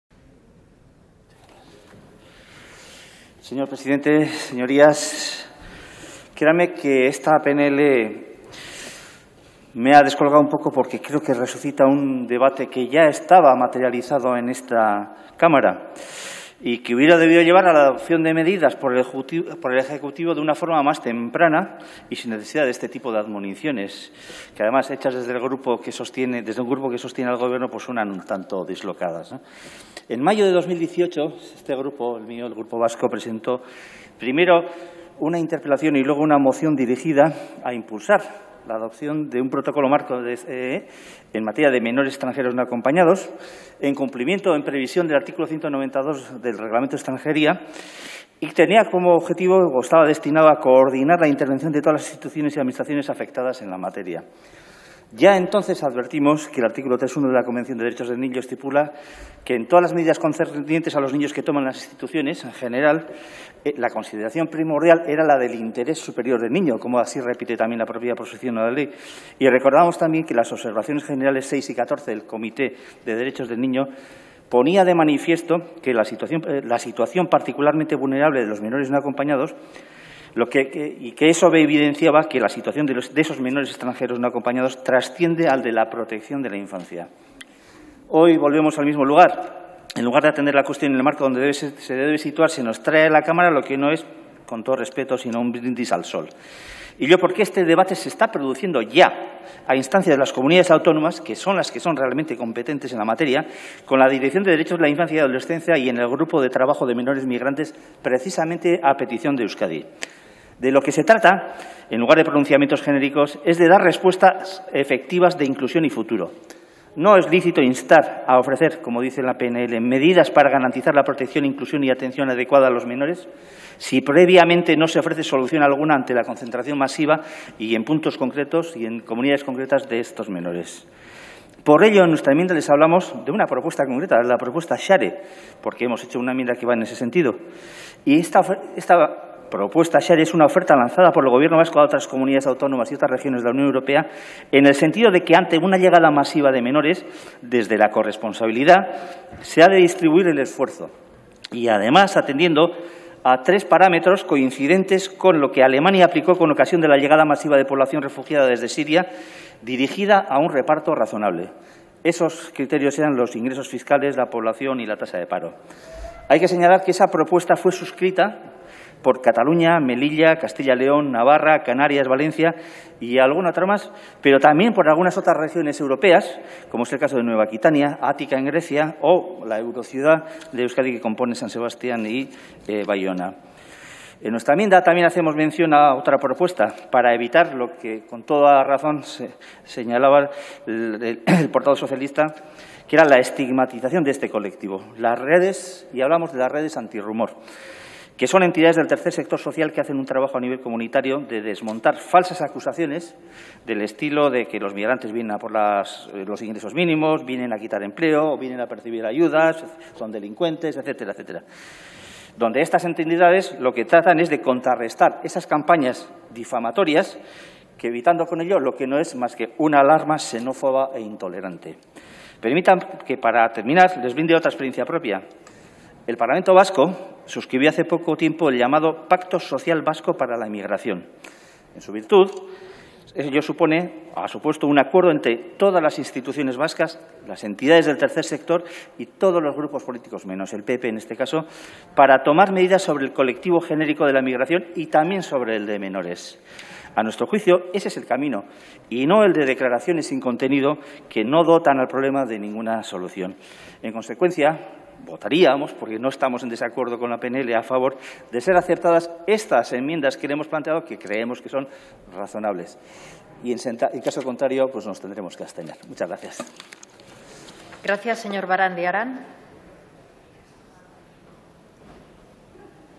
Intervención de Íñigo Barandiaran hoy 27 de Octubre 2020 sobre protección a los MENAS
Intervención de Íñigo Barandiaran Benito de EAJ-PNV, relativa a garantizar la protección, inclusión y atención adecuada de los y las menores migrantes no acompañados, el día 27 de Octubre de 2020.